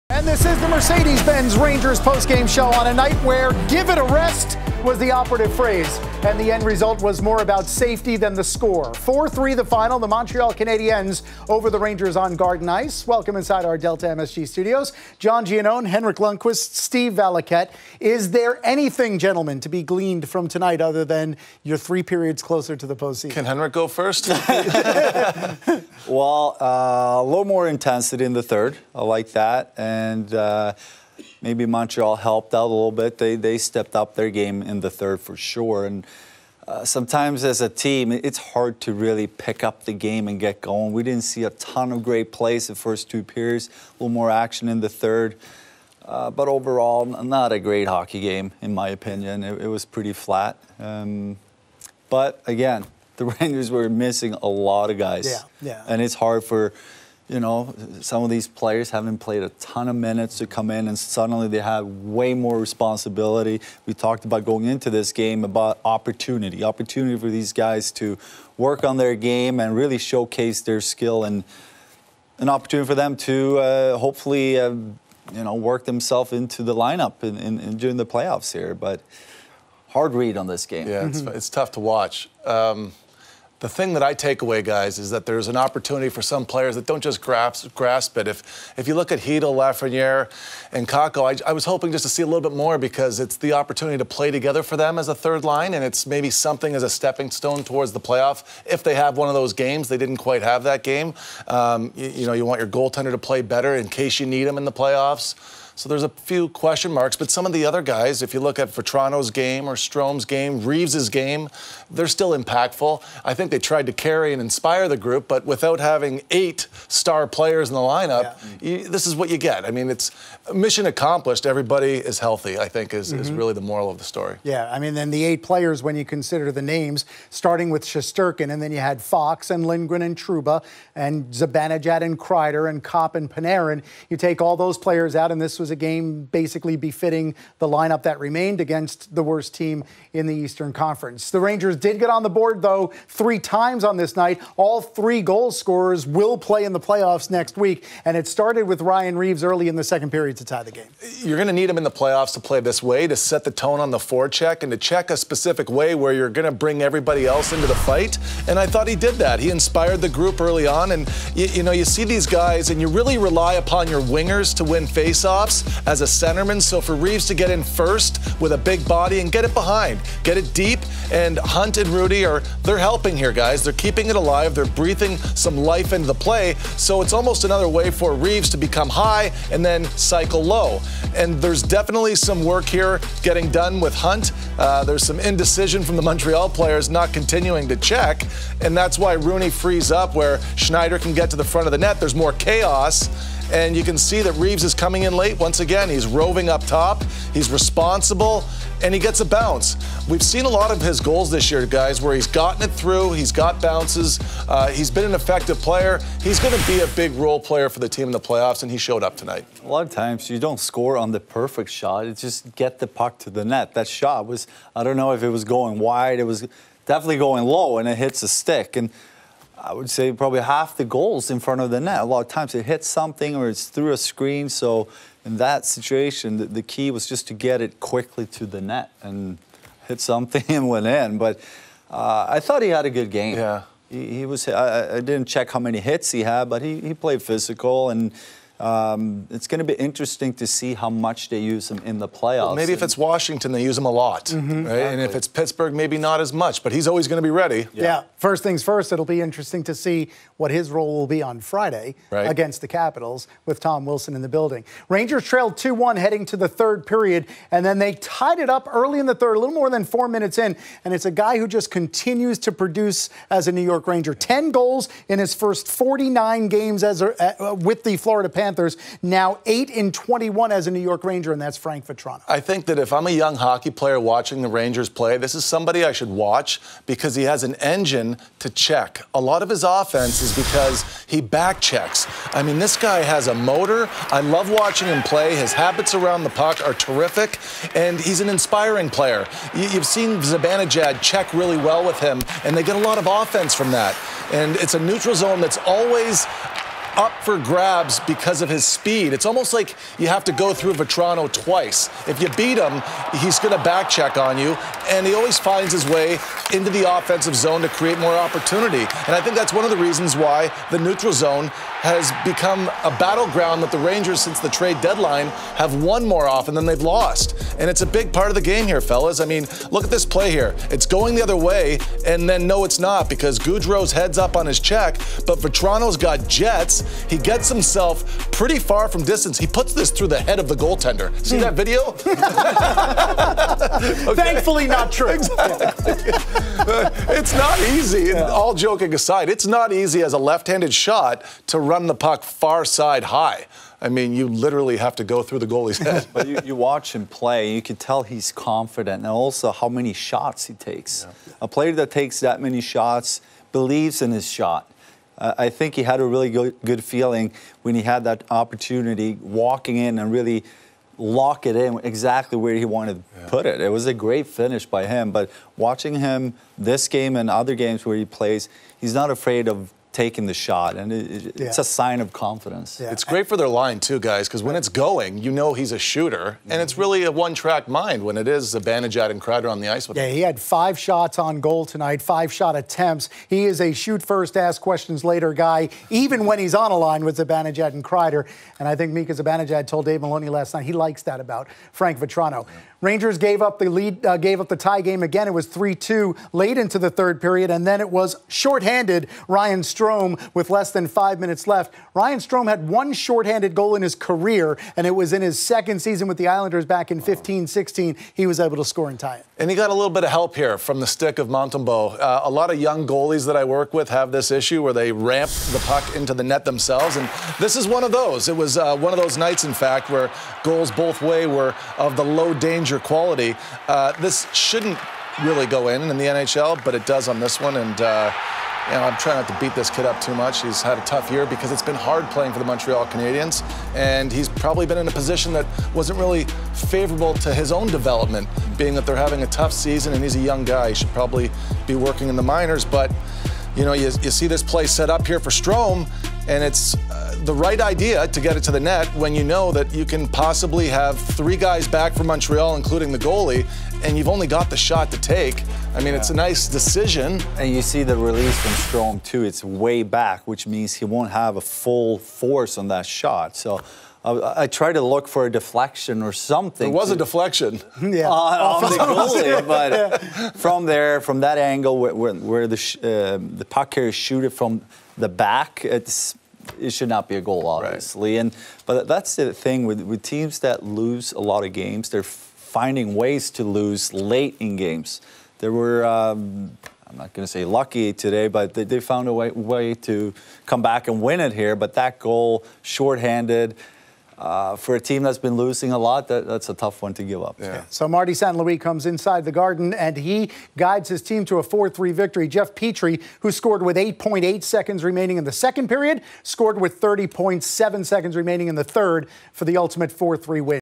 Rangers Fall 4-3 To Canadiens As Top Scorers Sit | New York Rangers
The New York Rangers lost their 2nd to last game of the season to the Montreal Canadiens 4-3. Ryan Lindgren, Adam Fox, Artemi Panarin, Andrew Copp, Mika Zibanejad, Chris Kreider and Jacob Trouba all sat out tonight as the playoffs approach. Ryan Reaves, Frank Vatrano and Ryan Strome each scored for the Rangers and Alexandar Georgiev had 20 saves.